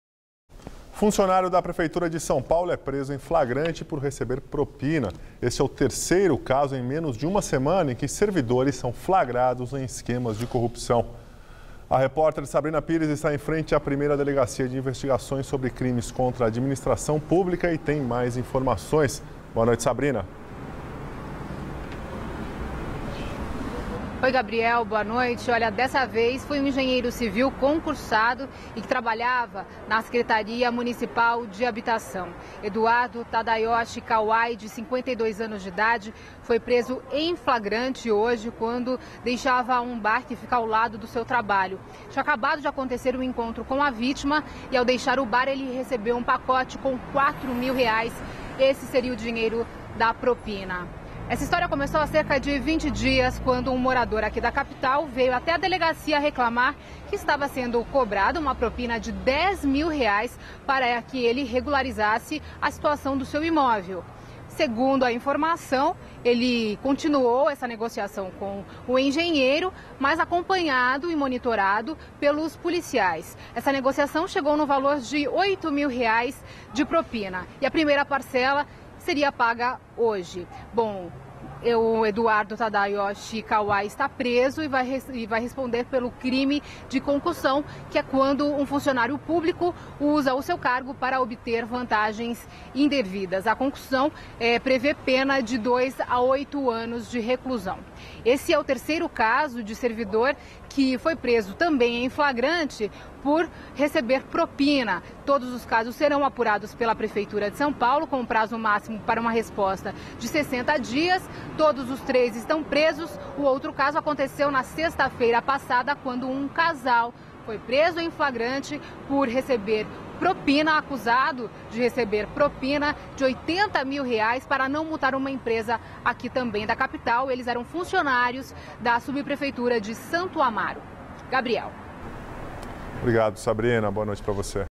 Servidor da Prefeitura preso por corrupção
Funcionário da prefeitura de São Paulo é preso em flagrante por receber propina. Este é o terceiro caso em menos de uma semana em que servidores são flagrados em esquemas de corrupção

Tema: Proprina